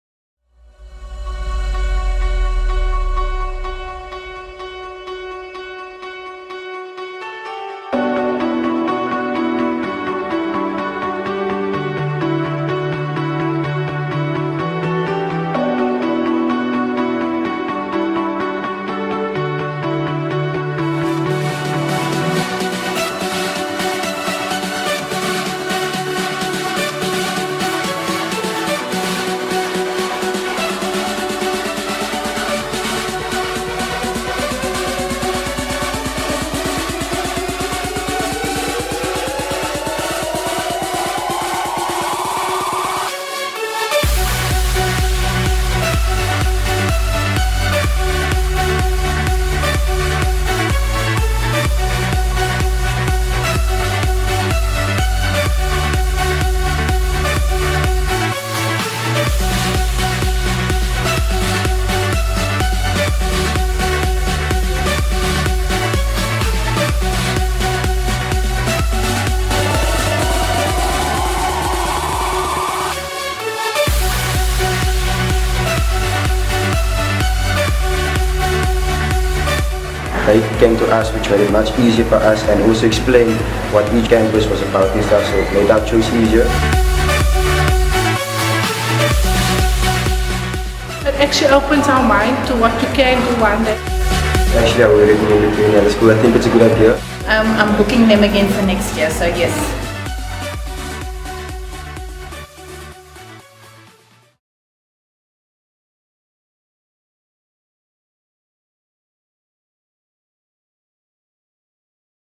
3RC Roadshow 2015 - Fairbairn college
Watch this entertaining and informative episode of the 2015 3RC roadshow as they visited Fairbairn college.